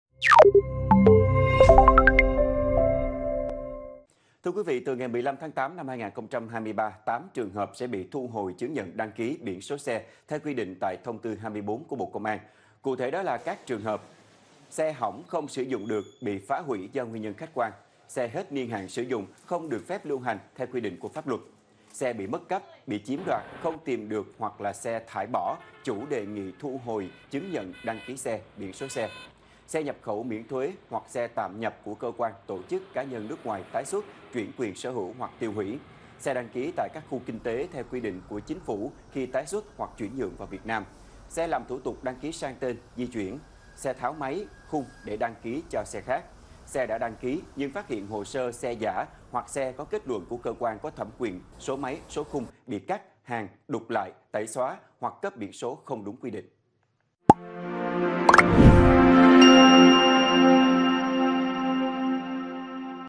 8 trường hợp sẽ bị thu hồi chứng nhận đăng ký, biển số xe từ 15/8 | Truyền hình Quốc hội Việt Nam
Truyền hình Quốc hội Việt Nam | Từ ngày 15/8/2023, 8 trường hợp sẽ bị thu hồi chứng nhận đăng ký, biển số xe theo quy định tại Thông tư 24 của Bộ Công An.